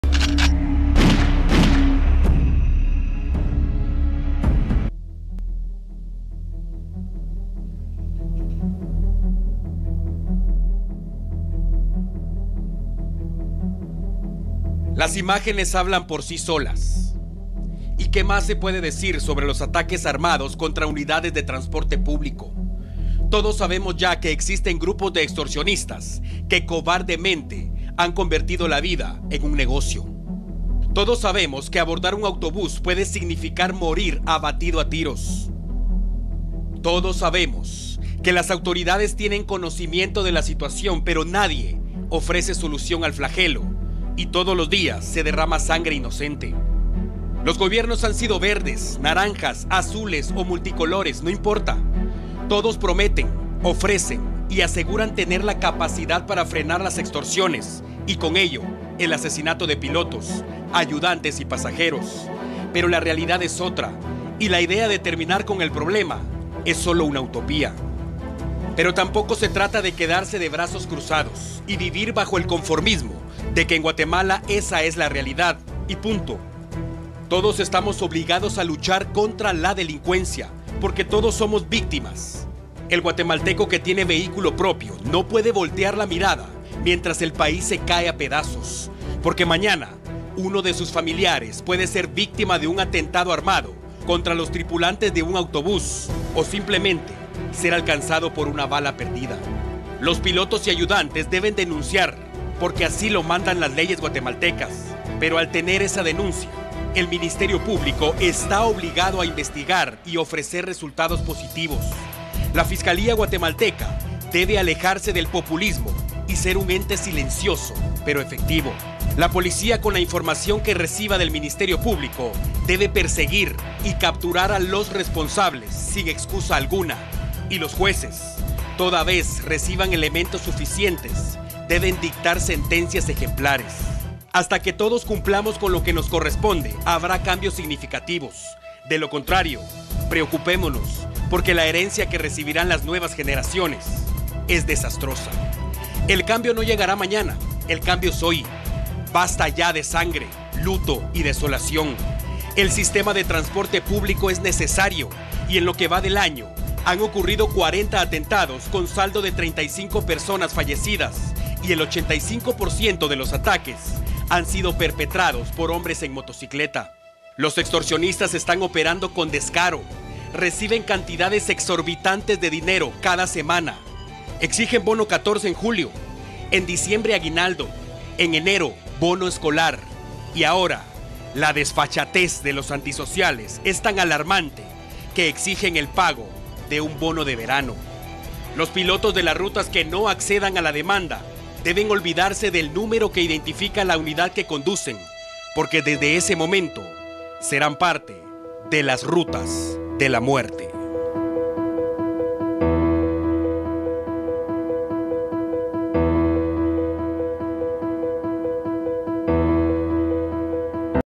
Extorsionistas piden "Bono de verano" a pilotos de autobús
En lo que va de año han ocurrido 40 atentados con saldo de 35 personas fallecidas. El 85% de los ataques han sido perpetrados por hombres en motociclistas.